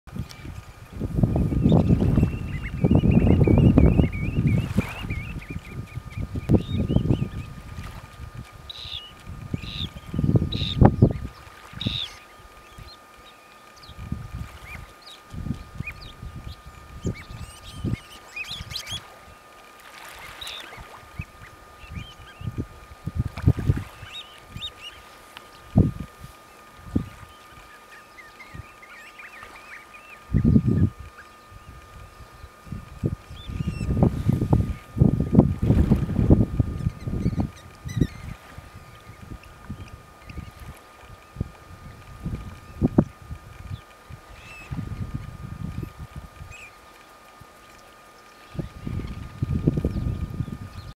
Eager foraging on the beach!
Juvenile "Icelandic" Black-tailed Godwit at Mølen, Norway on 5th Sept. 2013.